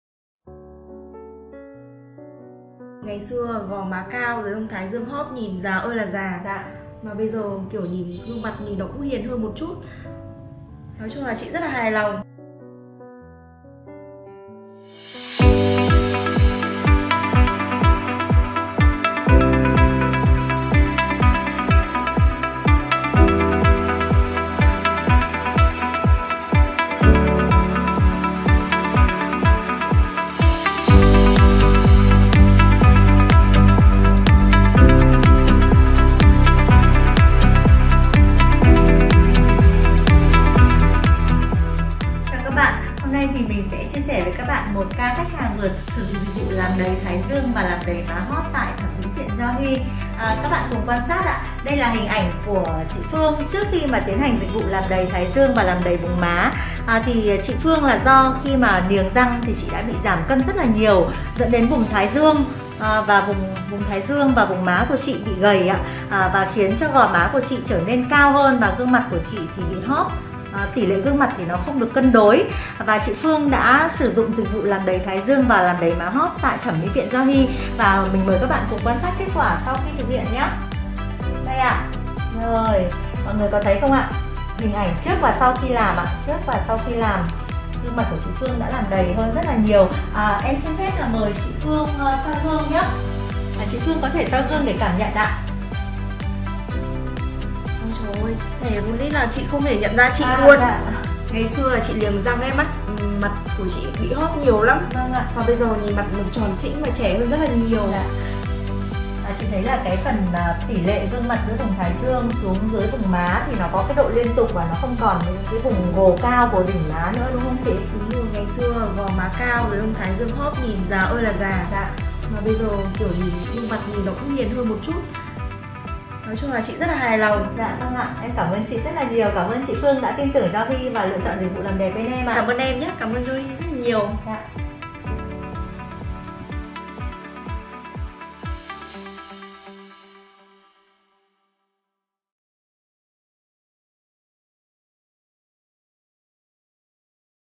Trẻ ra chục tuổi nhờ làm đầy thái dương và đầy má hóp
Theo thời gian, vùng thái dương hõm sâu, hóp má khiến khuôn mặt trở nên già nua, hốc hác. Một số người trẻ cũng có khả năng bị lõm thái dương hay hóp má do bẩm sinh hoặc do cấu trúc xương mặt của họ.

Với Yohe, chỉ 60 phút, thay đổi hoàn toàn khuôn mặt với công nghệ tái sinh tế bào Stemv Plus+, tạo hình đường nét hoàn hảo:

☀️ Lấp đầy phần thái dương và phần má hóp chỉ sau buổi đầu tiên.
☀️ Làm đẹp 4  không: Không xâm lấn - không đau đớn -  không tác dụng phụ - không cần nghỉ dưỡng
☀️ Công nghệ độc quyền chuyển giao trực tiếp từ Hàn Quốc.
☀️ Phác đồ chuẩn y khoa, được chính tay bác sĩ tu nghiệp nước ngoài thực hiện cho bạn.

Giải quyết hoàn toàn tình trạng hóp má hóp thái dương chỉ 60p ngay tại Yohe.
📍 Địa chỉ : 43A Giảng Võ, Phường Cát Linh, Quận Đống Đa, Hà Nội